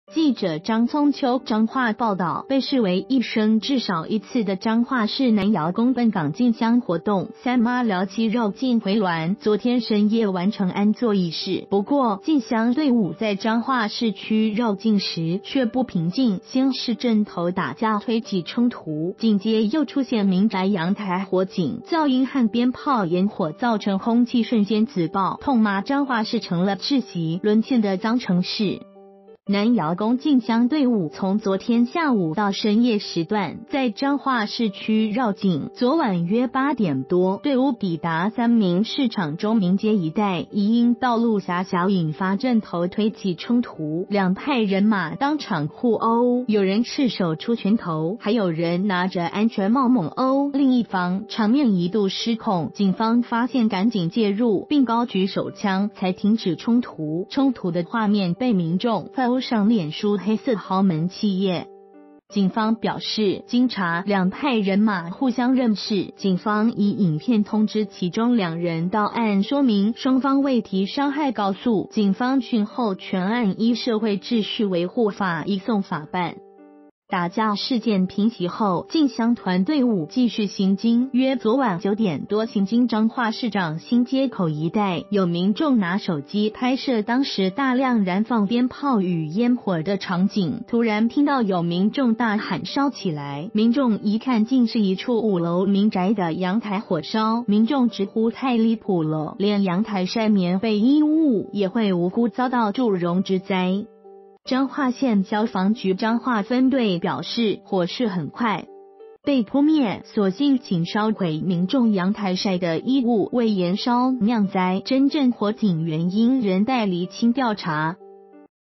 南瑤宮媽祖笨港進香回鑾 打架、火警先後來
〔記者張聰秋／彰化報導〕被視為「一生至少一次」的彰化市南瑤宮笨港進香活動，三媽潦溪遶境回鑾，昨天深夜完成安座儀式，不過，進香隊伍在彰化市區遶境時，卻不平靜，先是陣頭打架推擠衝突，緊接又出現民宅陽台火警，噪音和鞭炮煙火造成空氣瞬間紫爆，痛罵彰化市成了「窒息」、「淪陷」的「髒」城市。

發表評論評論如下。